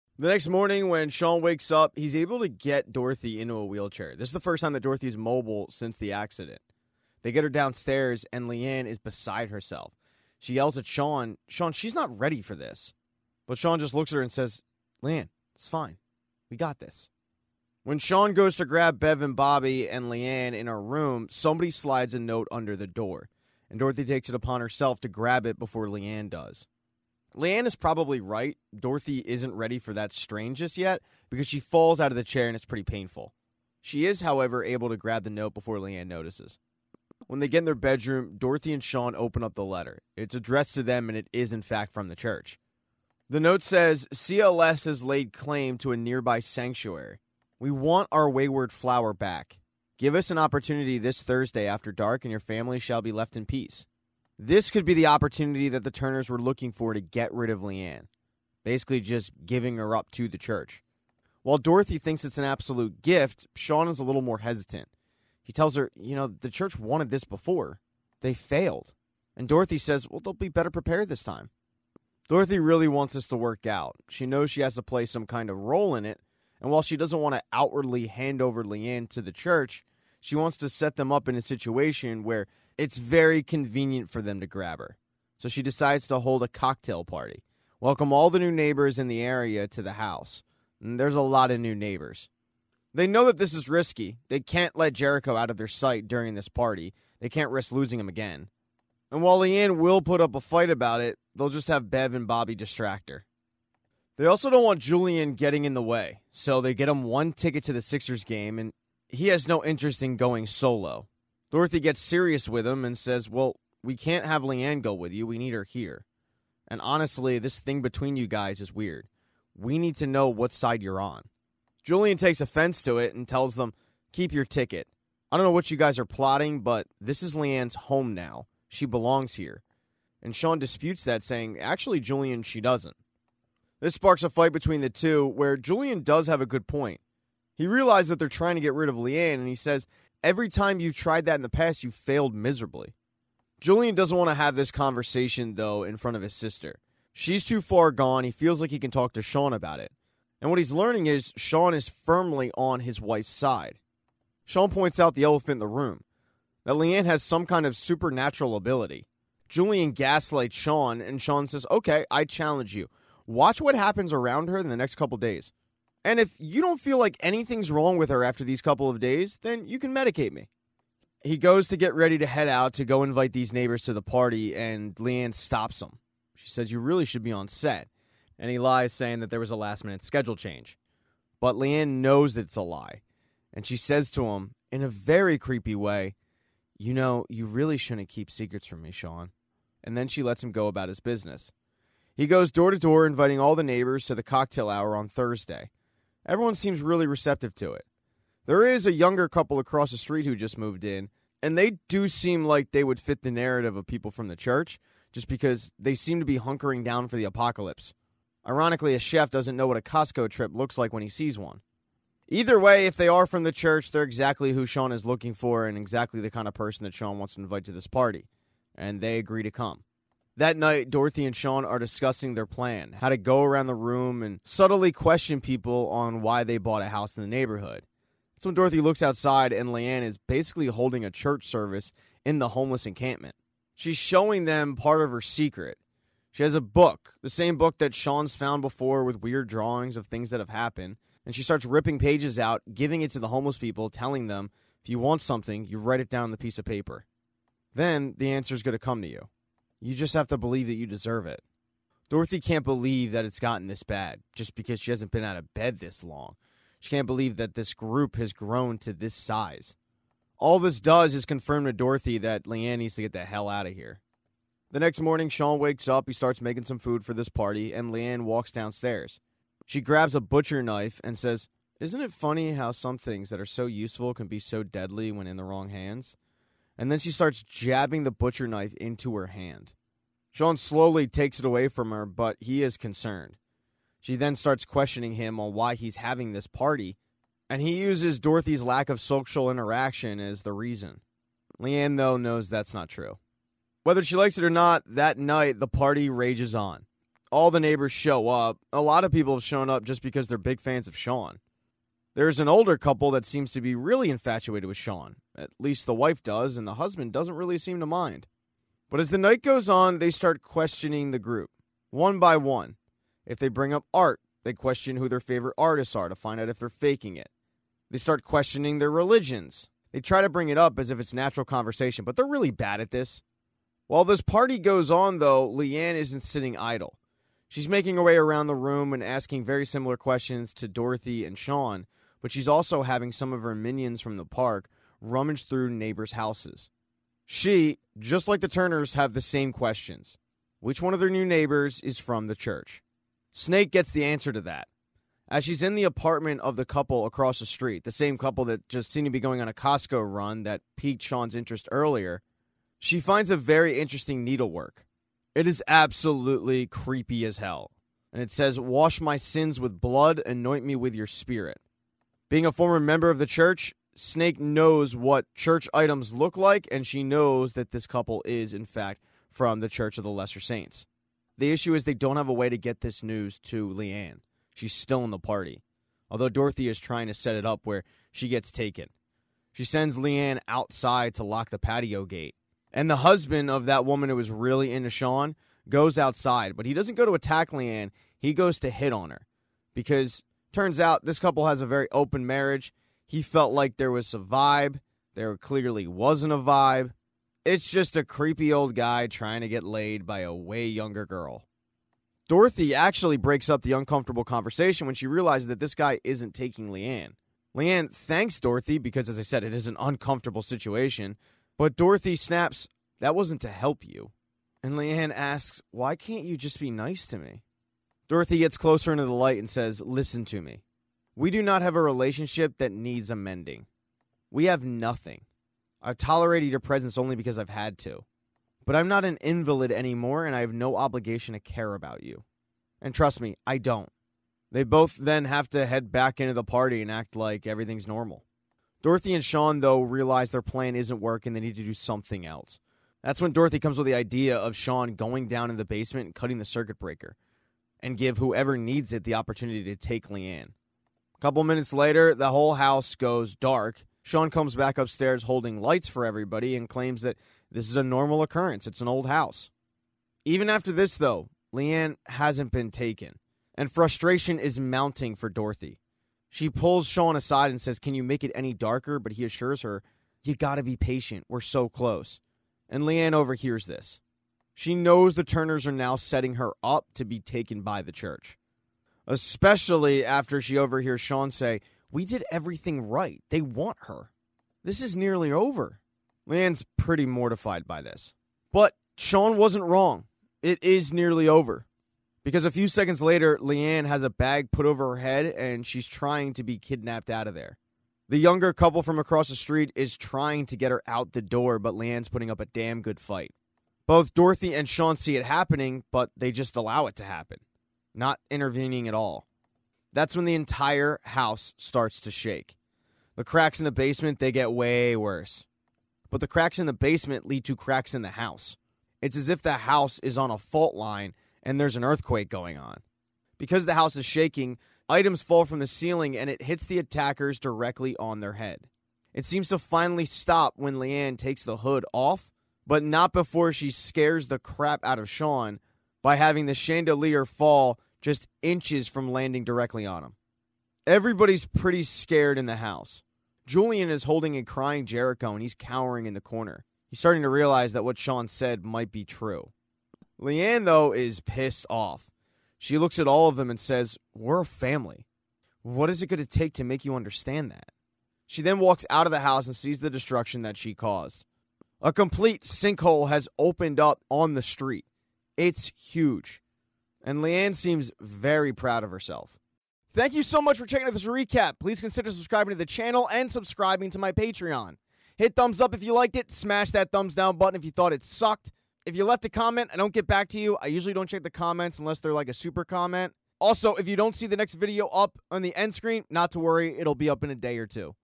Servant Season 4 Episode 5 Neighbors Recap
A recap of Servant Season 4, Episode 5. Neighbors

And check out my podcast Scene Invaders wherever you listen to podcasts